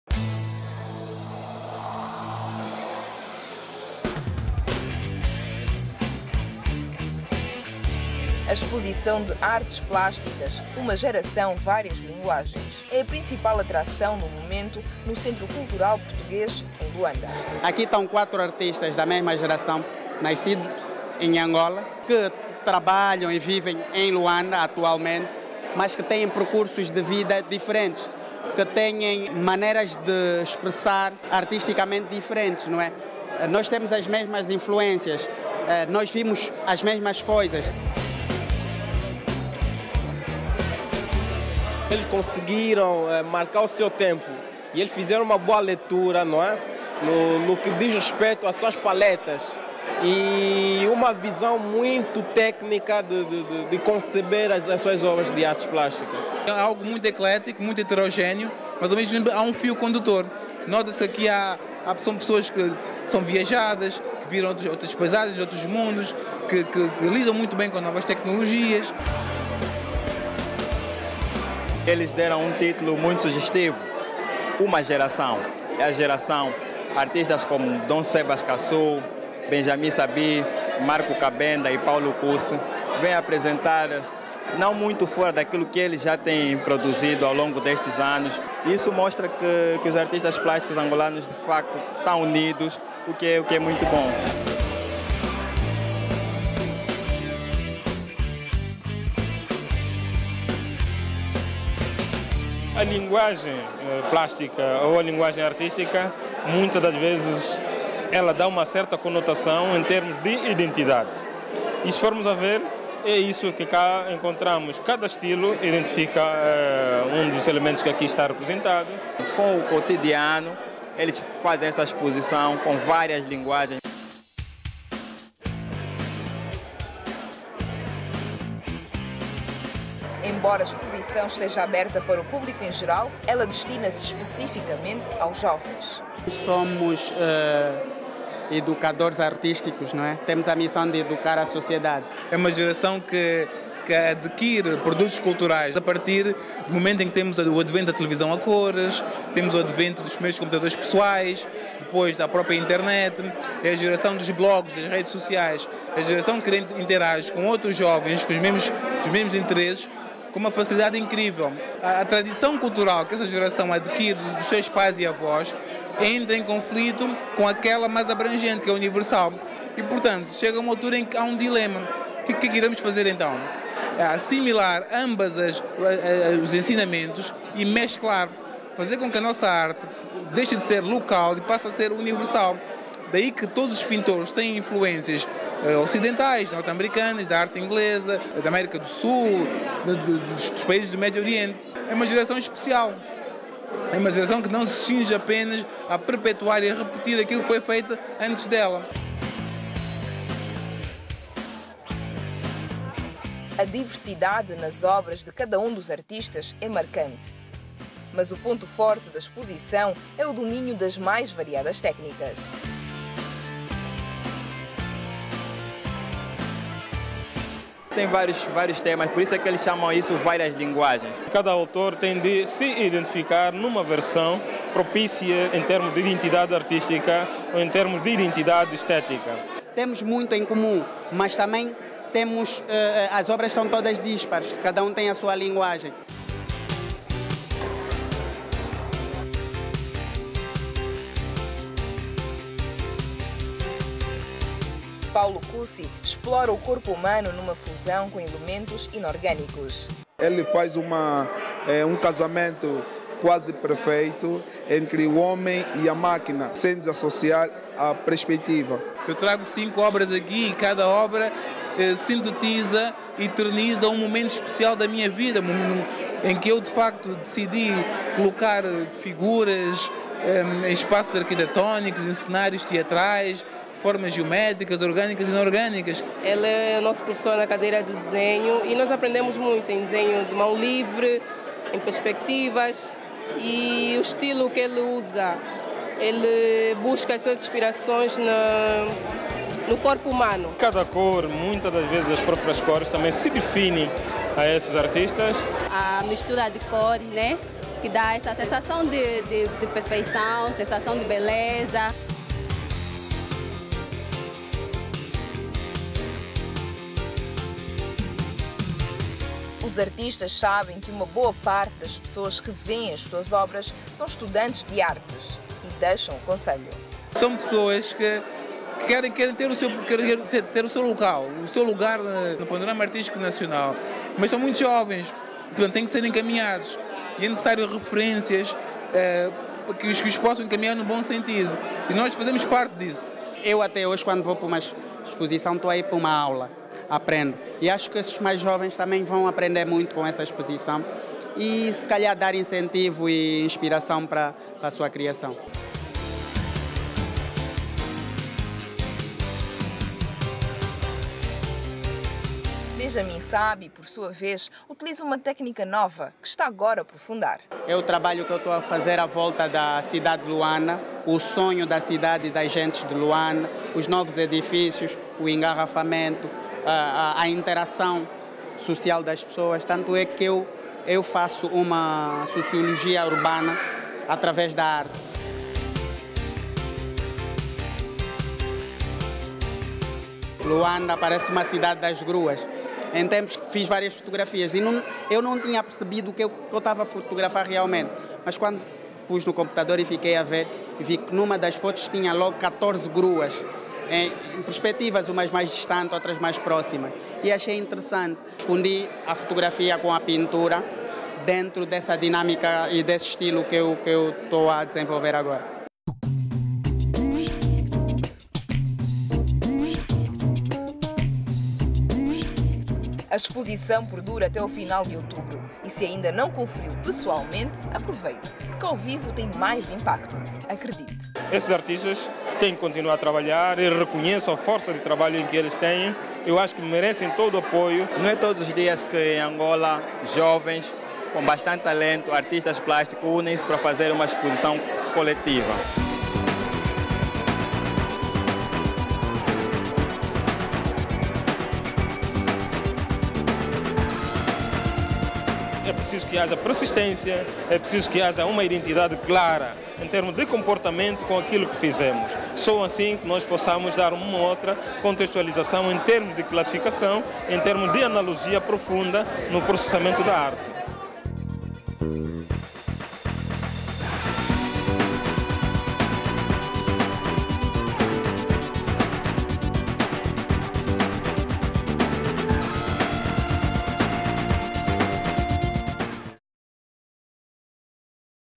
"Uma Geração - Várias Linguagens" - Expo Colectiva - Angola
Exposição Colectiva de Pintura com: Paulo Kussy, Benjamim Sabby, Don Sebas Cassule e Marco Kabenda.
Centro Cultural Português - Luanda / Angola